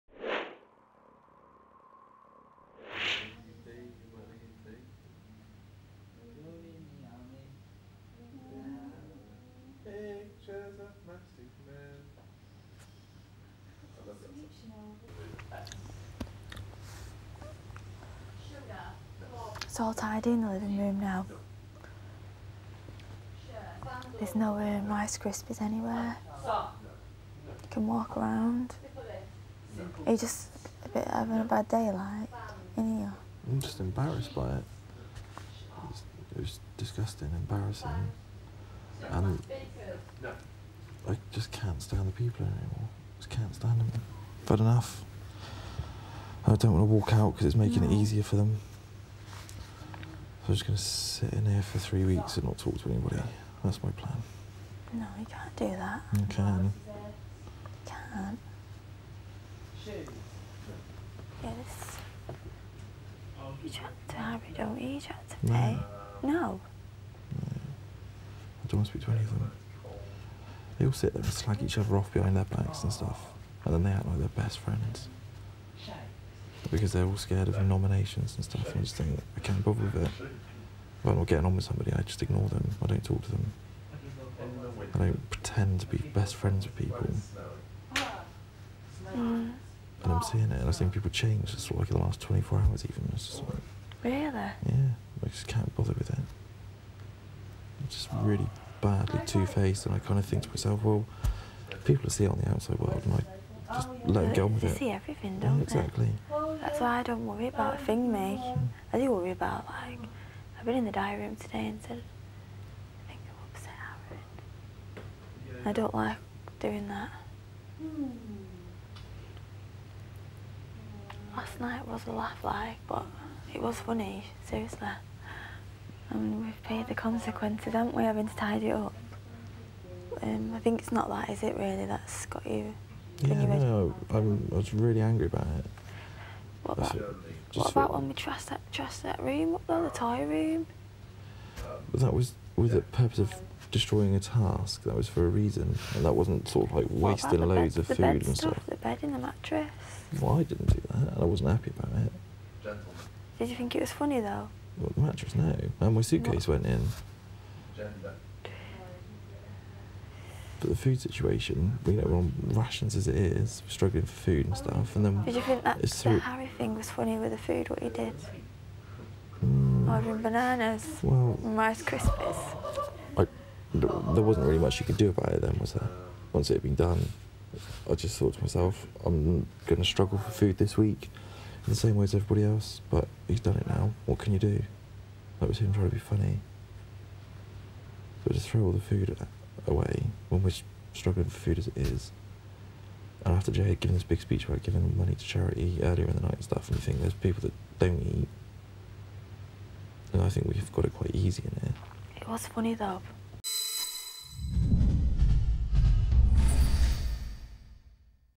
Day 37: Aaron, cereal-ously?
Louise tries to alleviate Aaron's bad mood by pointing out the funny side of last nights antics but he's having none of it.